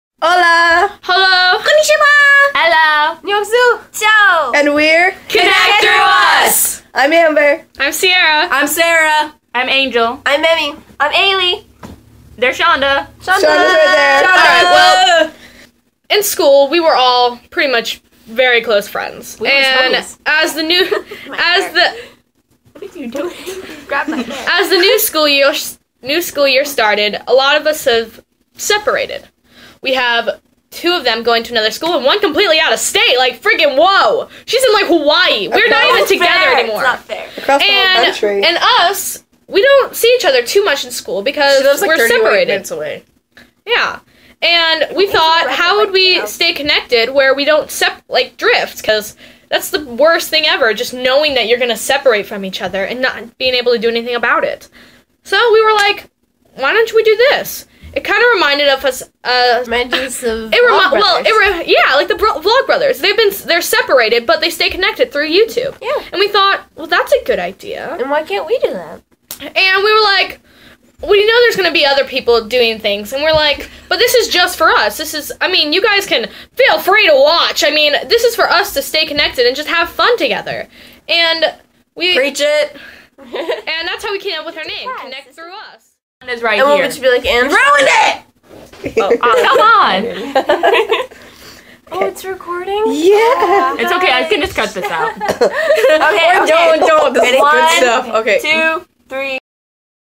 Connect Through Us Trailer
As our lives became progressively more busier we weren't seeing each other enough. Finally we made this channel. This is the start of ConnectThroughUs!